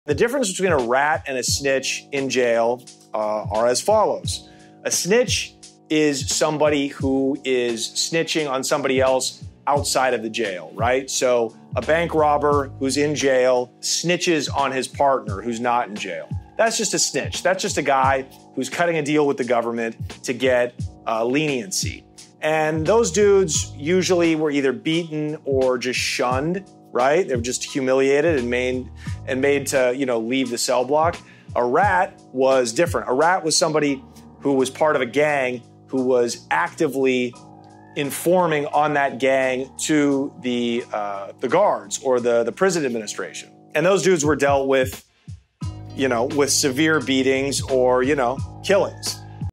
Former Prison Inmate Describes The Difference Between A Rat And A Snitch!
Former criminal Johnny Mitchell describes how rats and snitches are two different things in jail.